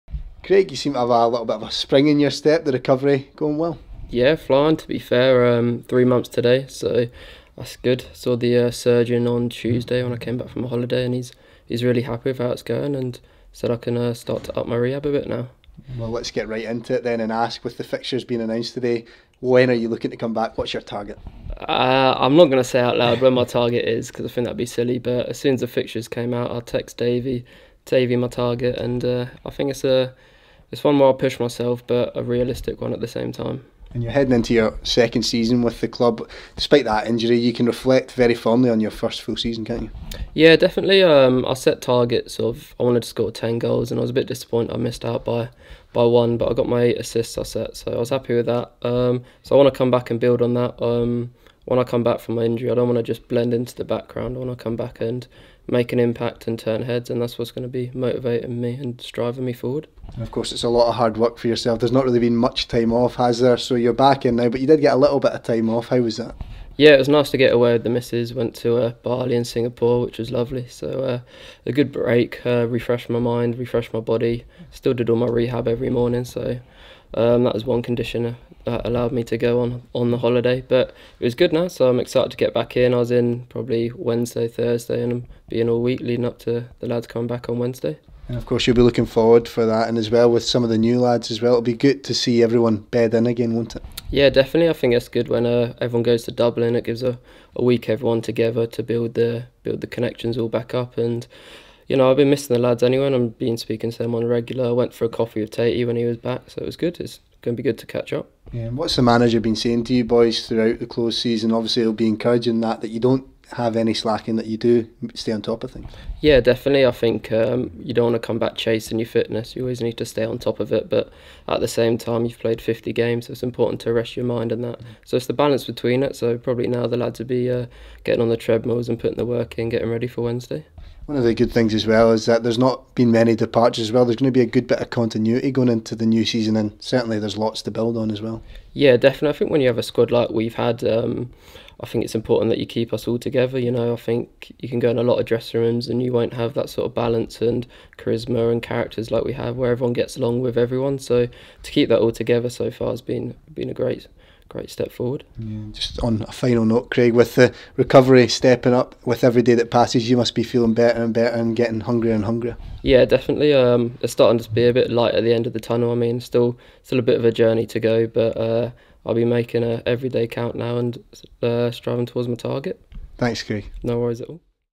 Craig Tanner steps up recovery
Forward Craig Tanner is closing in on his return to fitness as he steps up his recovery ahead of the 2018/19 campaign.